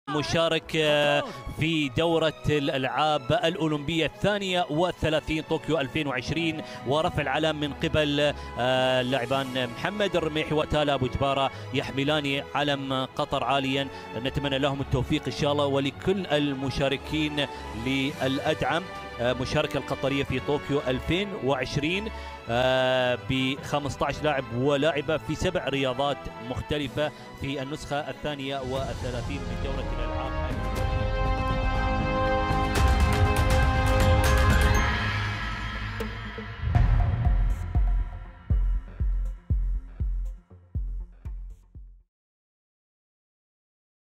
Team Qatar at the 2020 Olympics Opening Ceremony
Tokyo2020 is the first Olympics edition to see two athletes carry their country's flag.

Trap shooter Mohamed Al Rumaihi and Rowar Tala Abujbara have been named TeamQatar's flagbearers in the Olympic Games opening ceremony.